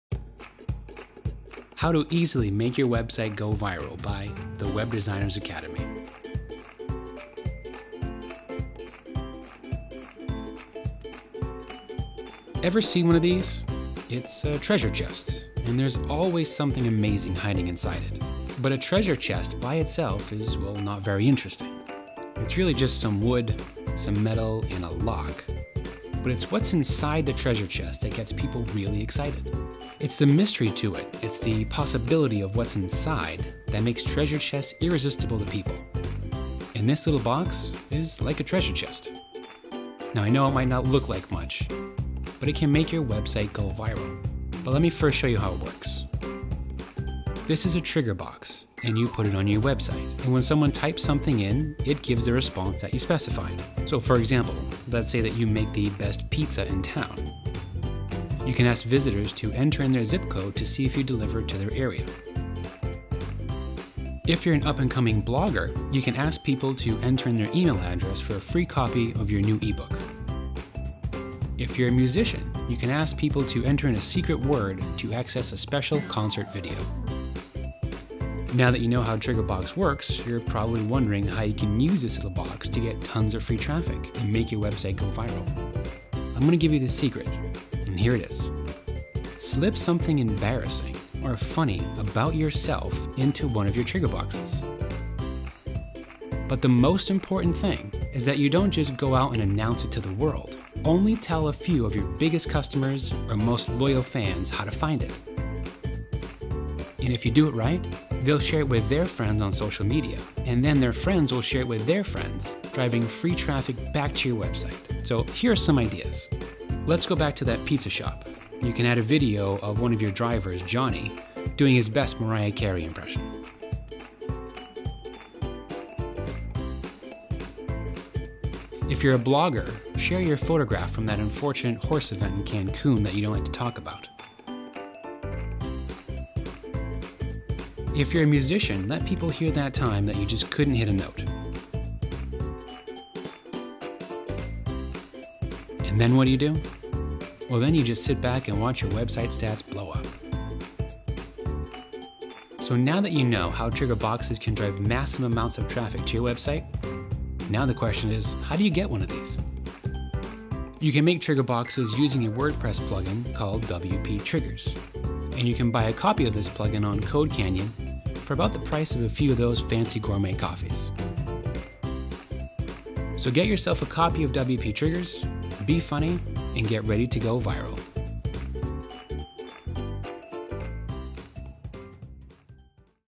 You're doing it wrong... how to easily make your website GO VIRAL
If you've been struggling to get free traffic to your website, this video will show you how! It's easy to do and can be used for other things (like a zip code search OR to collect email addresses).

Going viral only requires you to give people something to share with their friends and this is the best way to do it!